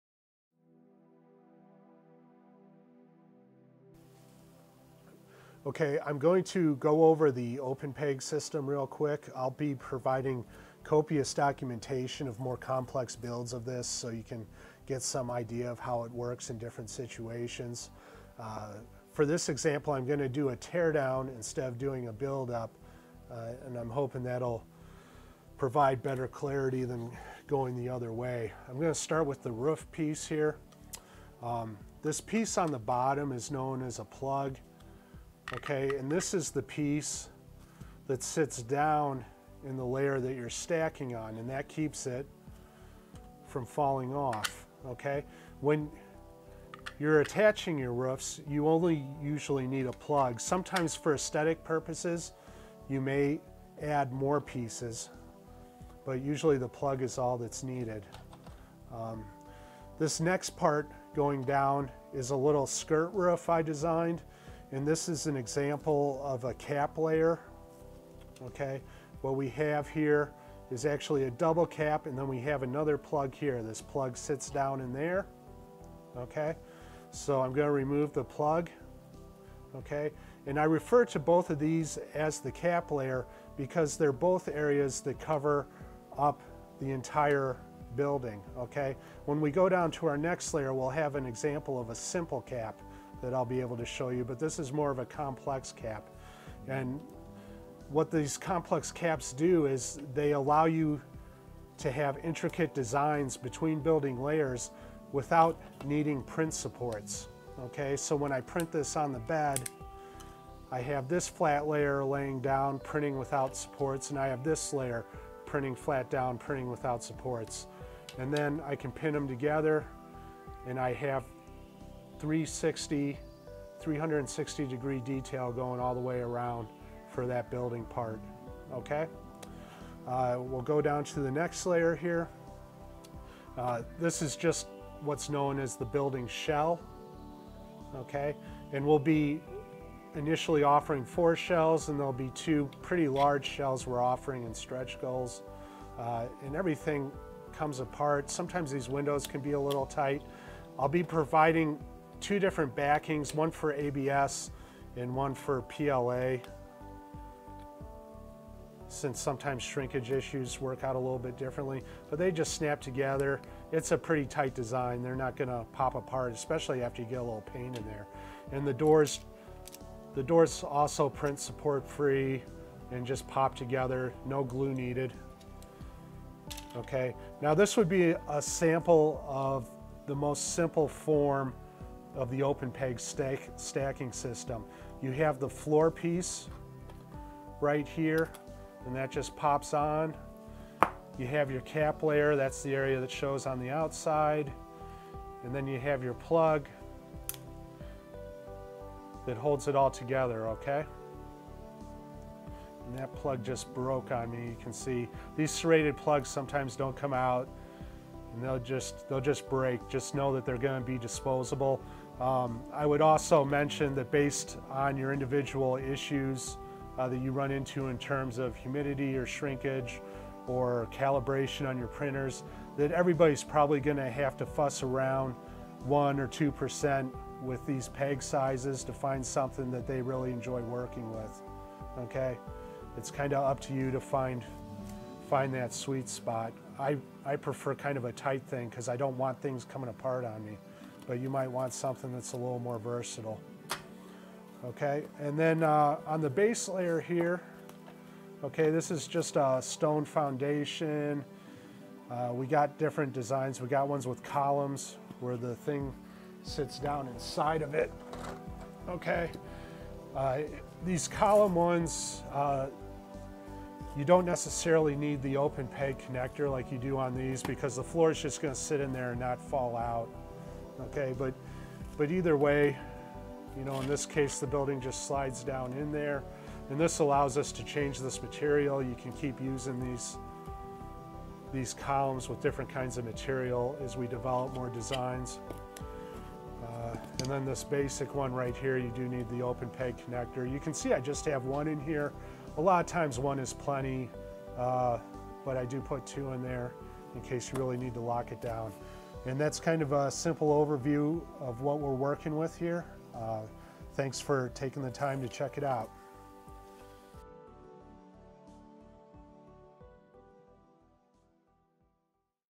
OpenPeg Teardown
OpenPeg Explained! See our teardown and explanation of pegs, plugs, caps, walls, windows, and doors used to design infinite 3D printable building options.